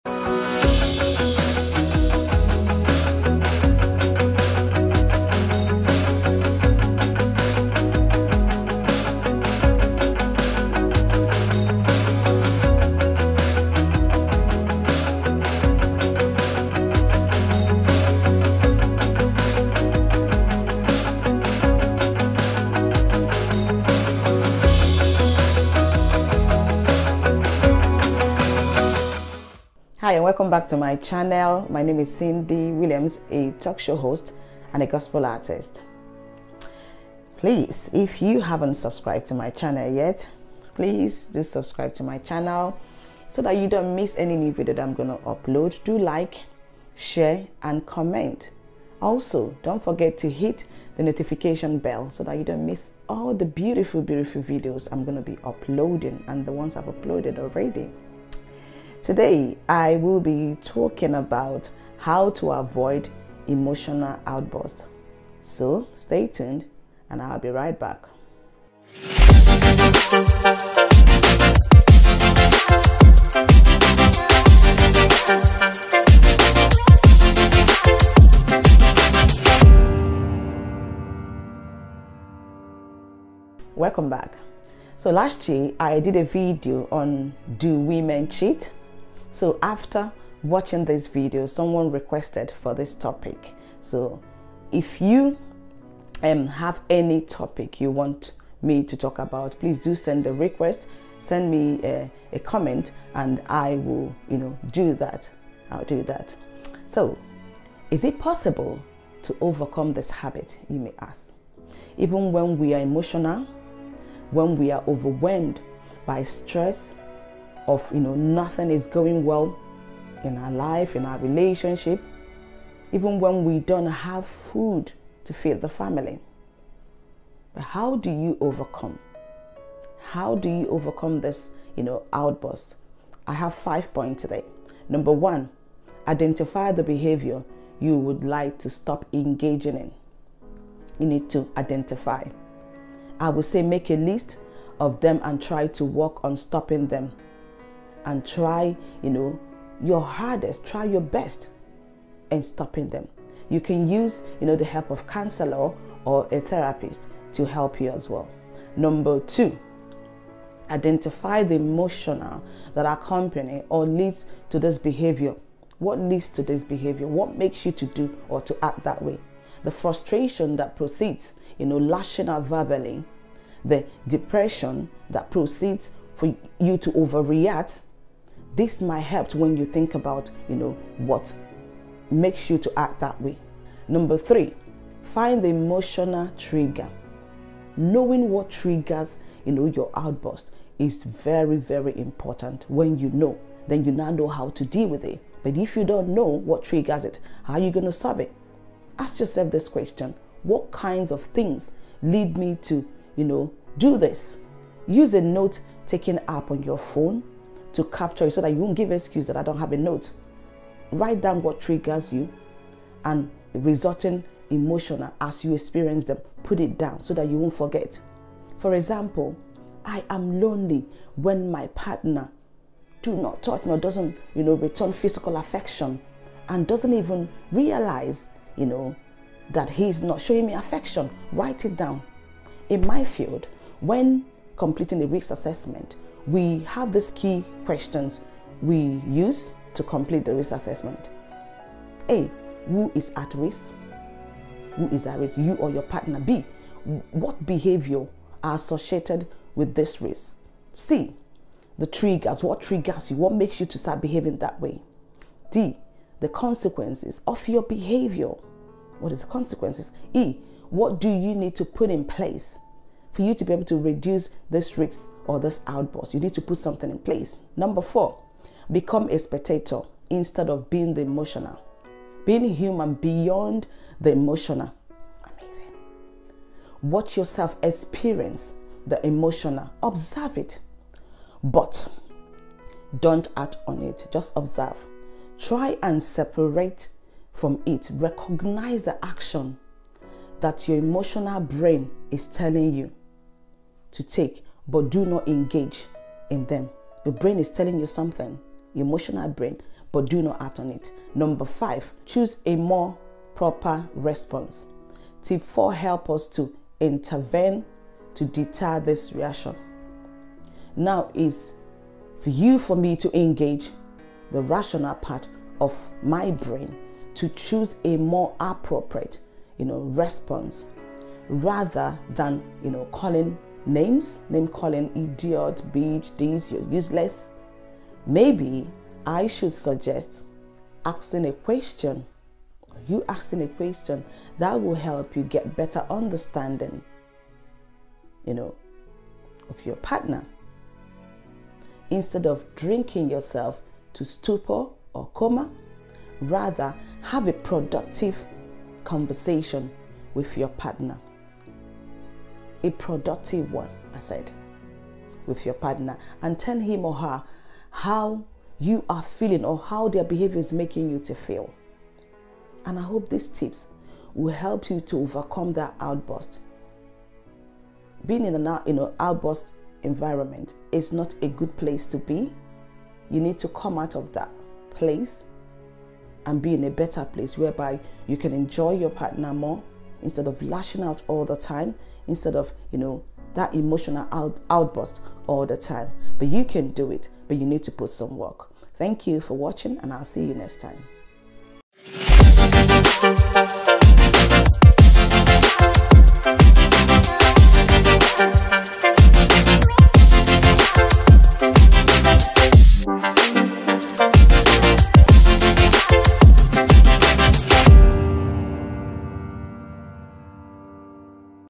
How to avoid an emotional outburst
Never strike out of anger if at all possible, you own your feelings. you own your thoughts and you can control both.

Please do watch: Do women cheat?